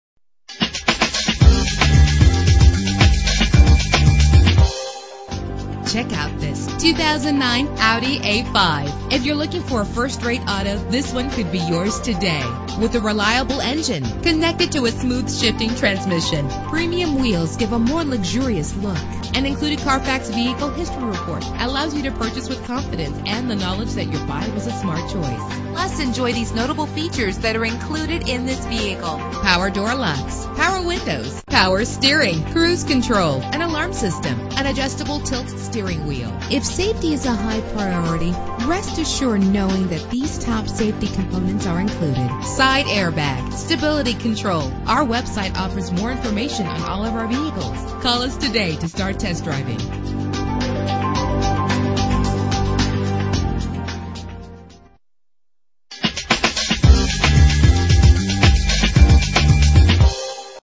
2009 Audi A5 - Manheim PA
Year: 2009
Make: Audi
Model: A5
Trim: 3.2 Quattro Coupe 2D
Engine: 6 cylinder 3.2 Liter
Transmission:
Color: Black
Mileage: 49184
Address:
14 Anthony Drive